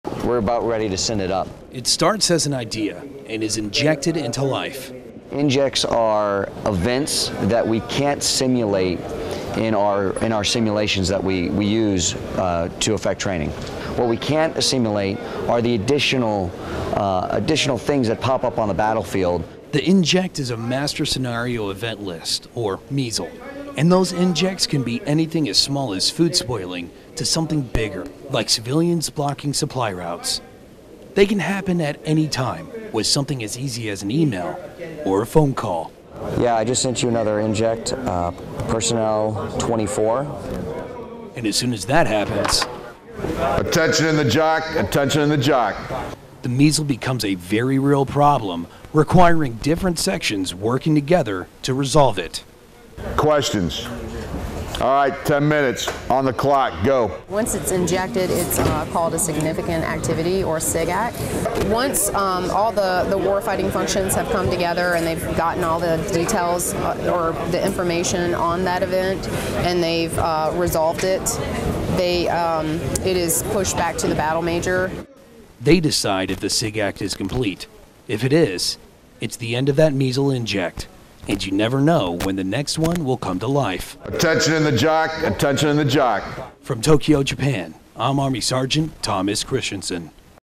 YS - The Life and Death of a MSEL
This is Yama Sakura 67, a bilateral command post exercise between Japan and the United States.  This training event tries to cover everything possible to improve combat readiness, but it can't get everything.  When that happens, they send in a MSEL.